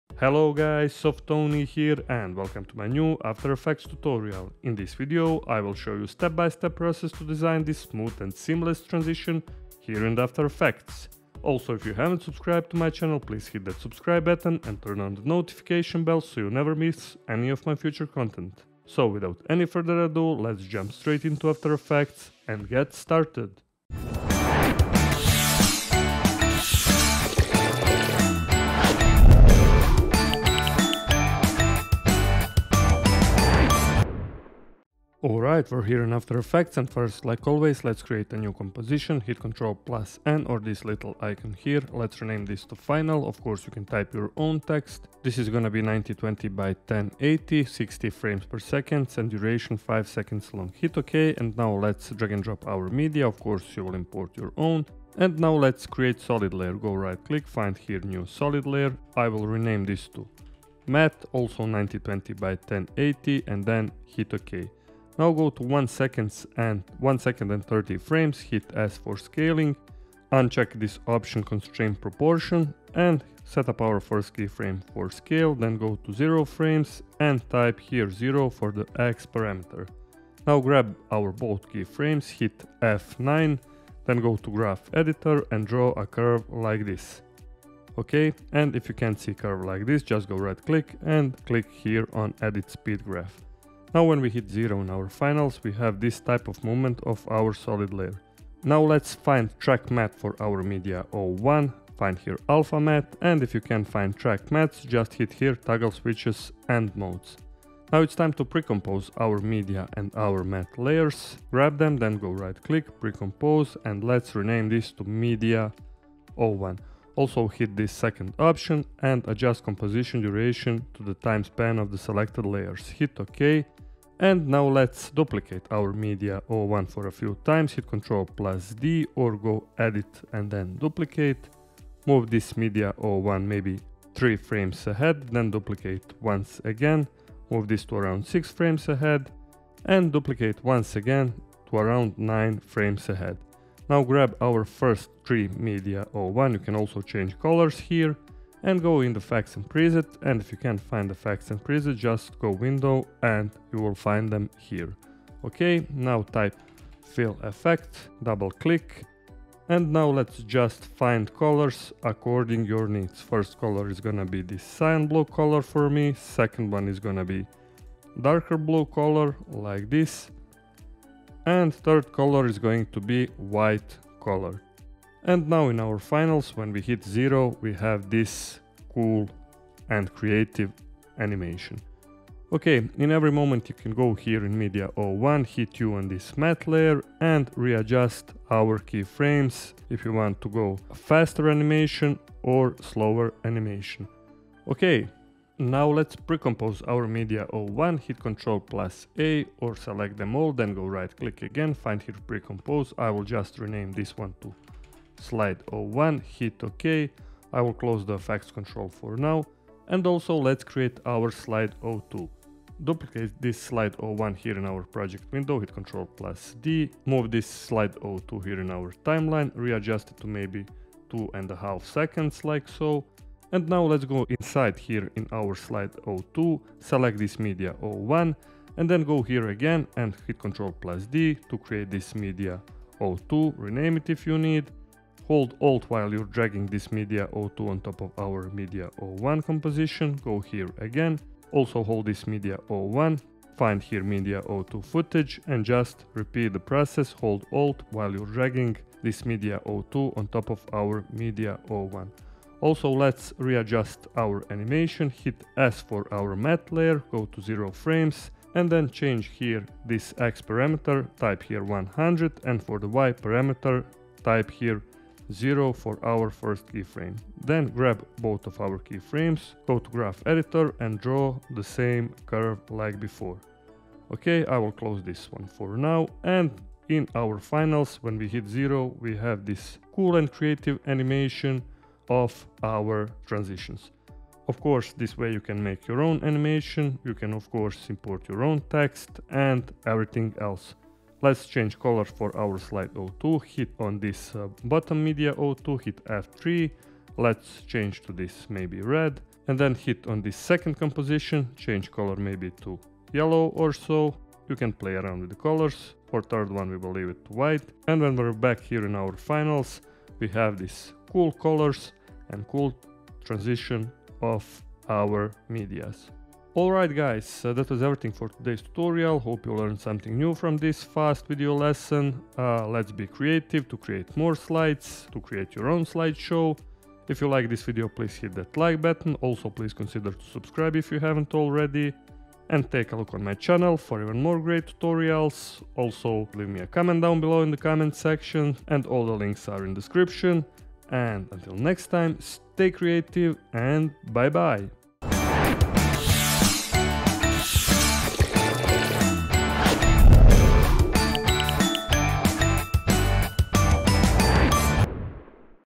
LINEAR TRANSITION in After Effects - After Effects Tutorial
Hey everyone, in this after effects tutorial I will show you how to create cool and simple linear transition for your own purpose - your slideshows, intros, promos, lower thirds in After Effects without using any plugins. This technique is a cool way to make your videos or images looking even better and to impress your audience. Very easy to make, just follow my steps!

*** My Portfolio - Stock Sites ***

*** Do you like music - Check out Senchi's portfolio ***